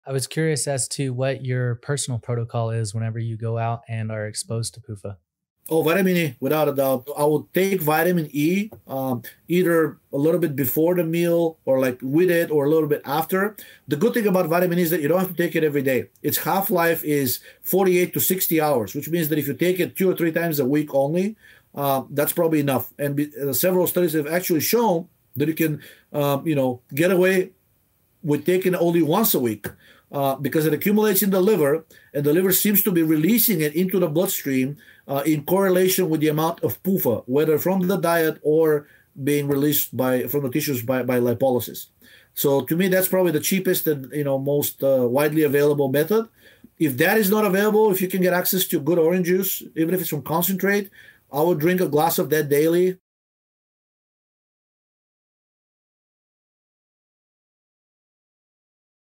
Georgi Dinkov’s Eating Out Protocol
Georgi shares his personal protocol on what he does when he goes out to eat a less than ideal meal